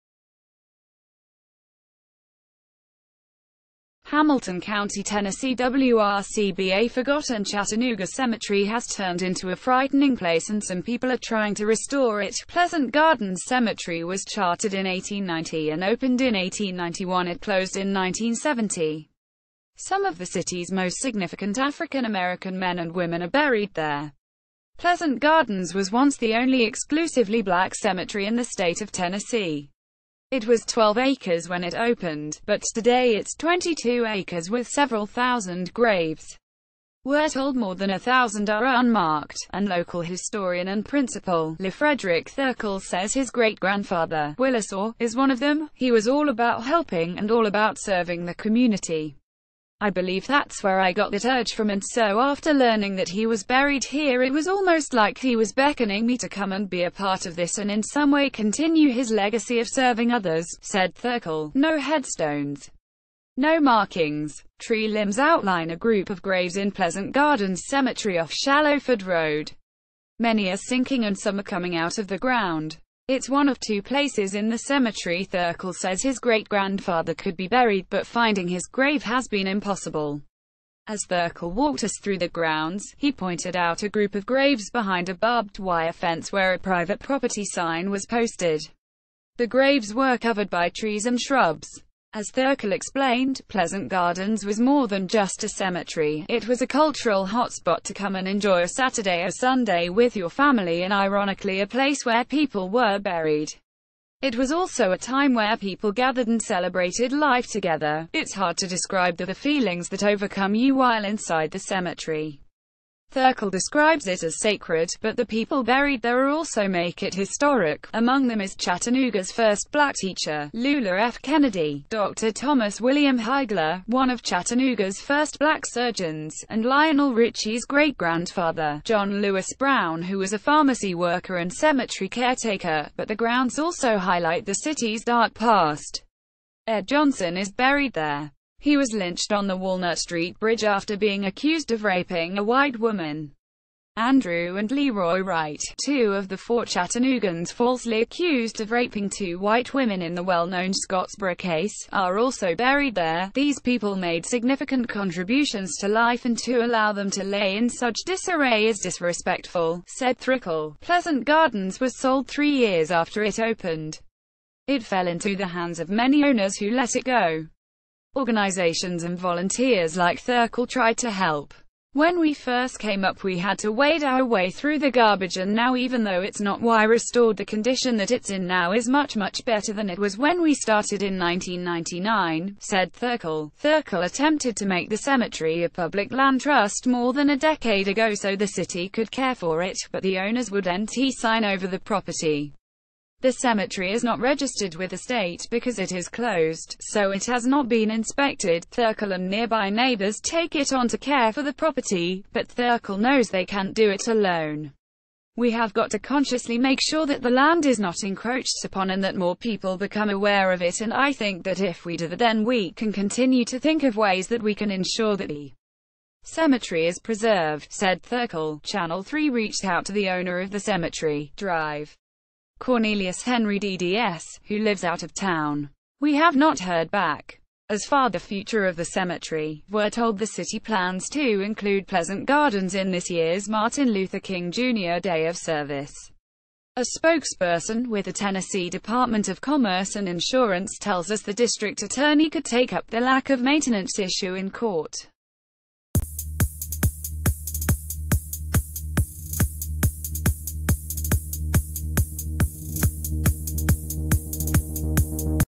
Community members trying restore forgotten cemetery | UK News Express
Community members trying restore forgotten cemetery
HAMILTON COUNTY, TN WRCB A forgotten Chattanooga cemetery has turned into a frightening  place and some people are trying to restore it. Pleasant Gardens Cemetery was chartered in 1890 and opened in 1...